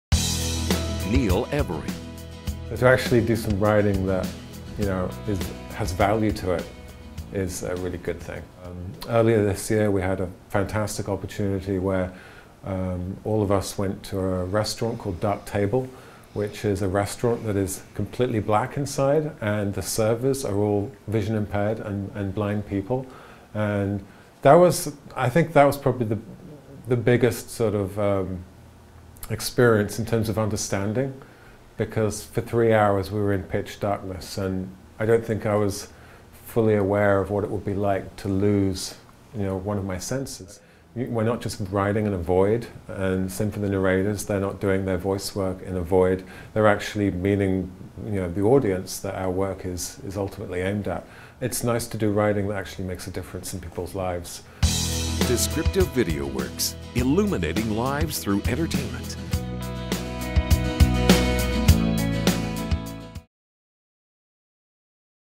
Descriptive Video Works - Neil
Meet Neil Every who is a writer at Descriptive Video Works.

For more information about our audio description services, call us Toll Free at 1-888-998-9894